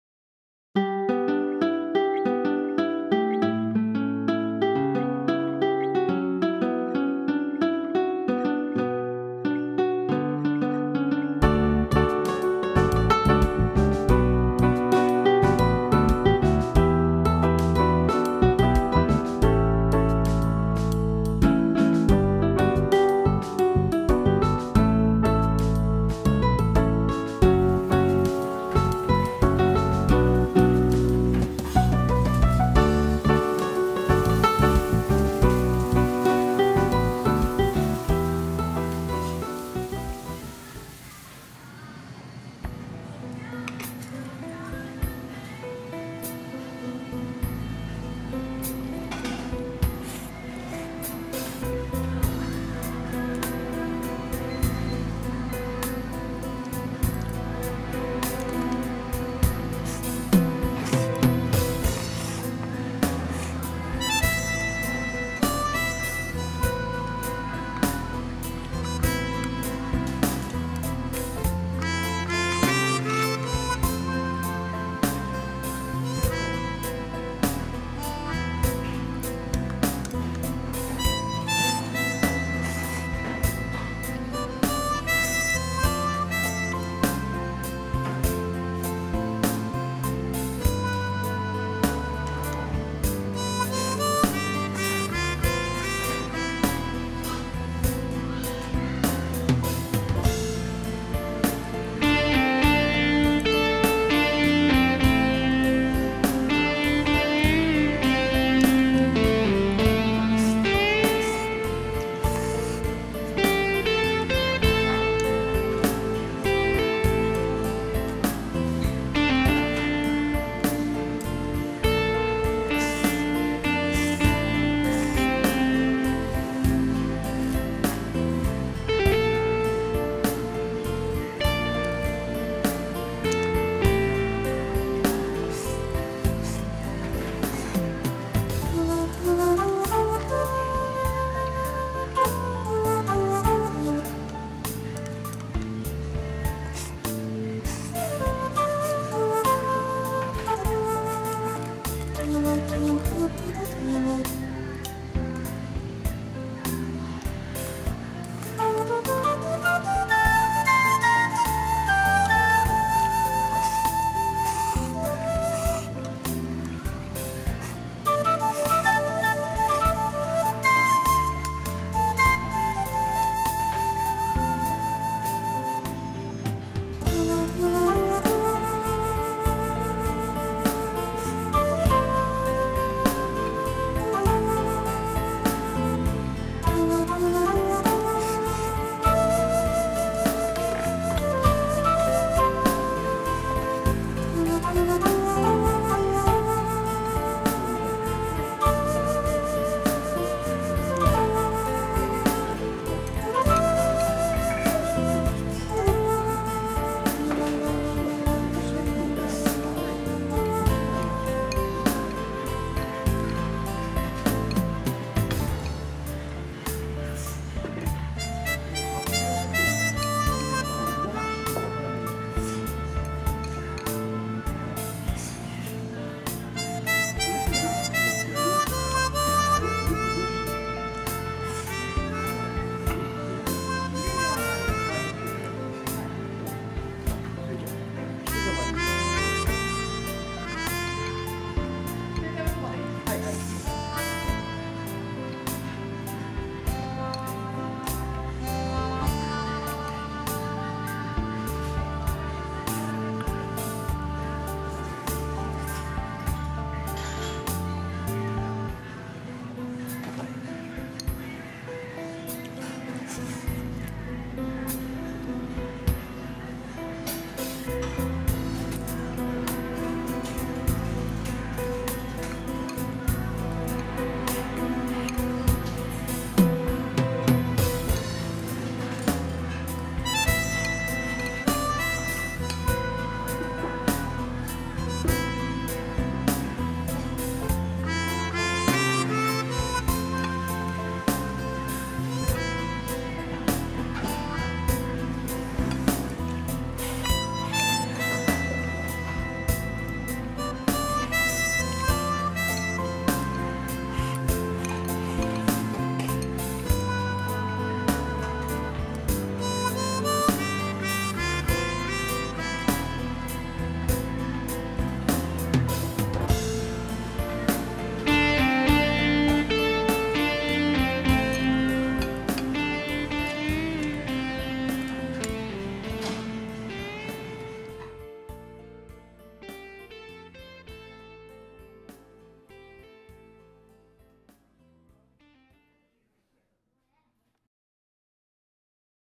【ウエスト④】 新そばと鶏炭火焼親子丼を食べて来ました ＃651
以前食べたウエストのそばが新そばがしんかしてました。